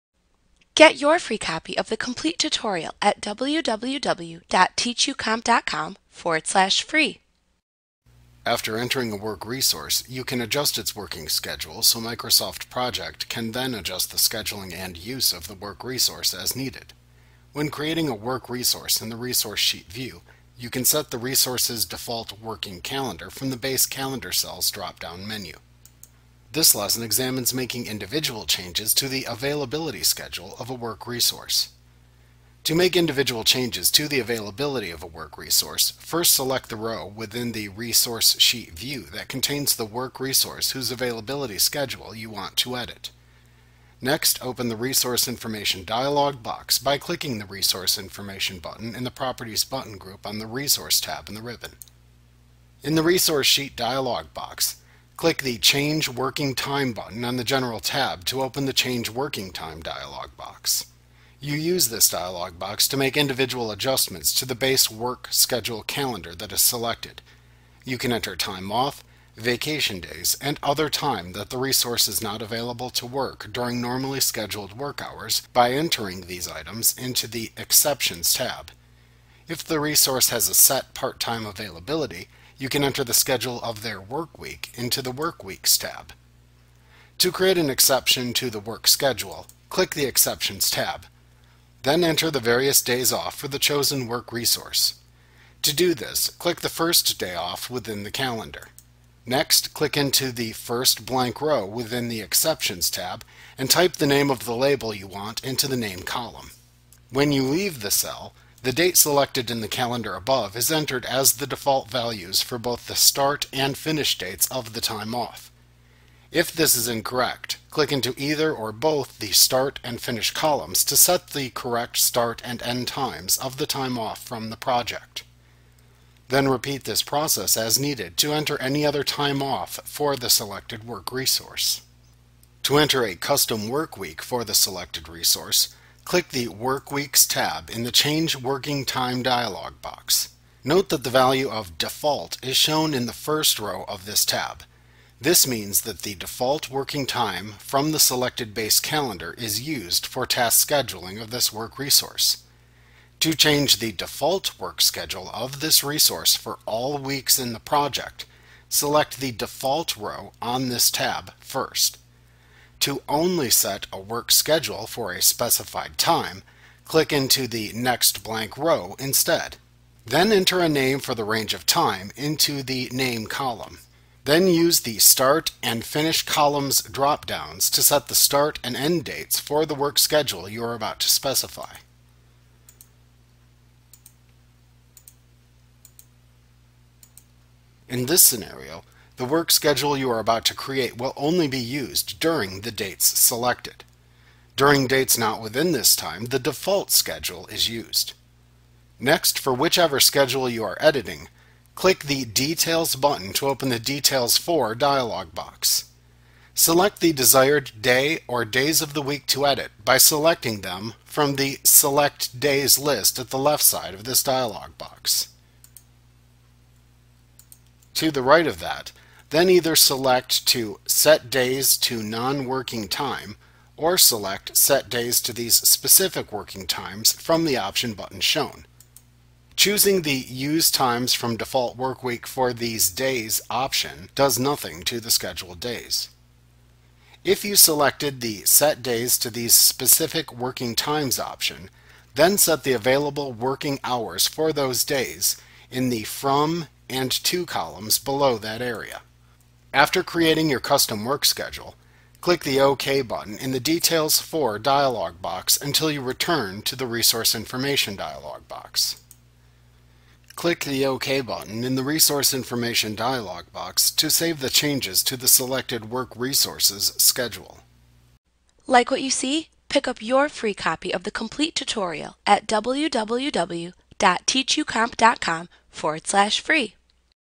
Project 2016 Tutorial Scheduling Work Resources Microsoft Training
Learn how to schedule work resources in Microsoft Project A clip from Mastering Project Made Easy v. 2016. - the most comprehensive Project tutorial available. Visit us today!